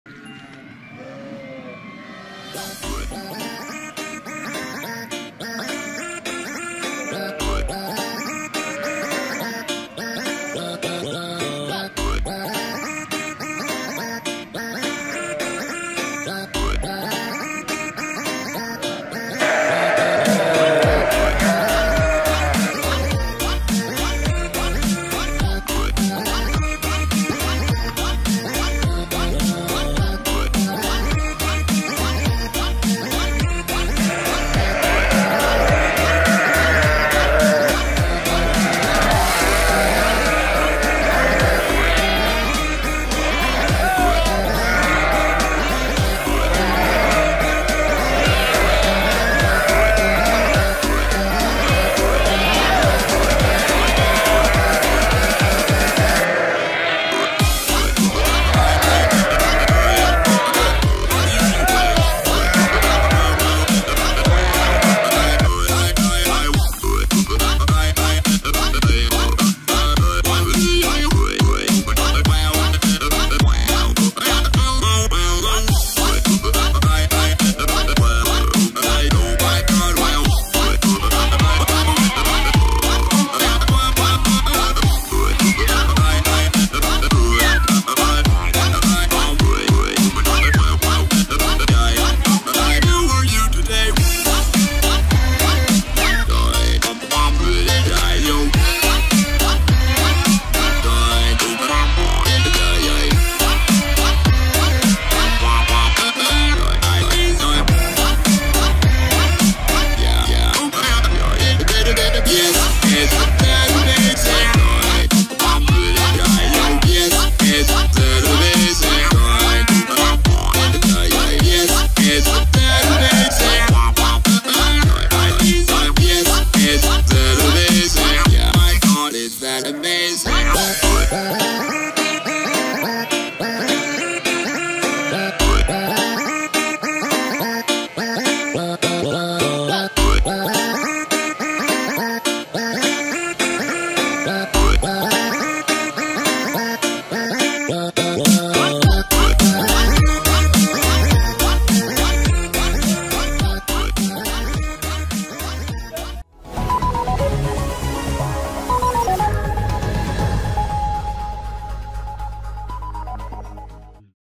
Meseleye Giden Elazığlı Kuzular
Deprem felaketinin yaşandığı Elazığ’da baharın habercisi kuzuların anneleri ile buluşması hem yürekleri ısıttı, hem de renkli görüntüler ortaya çıkardı.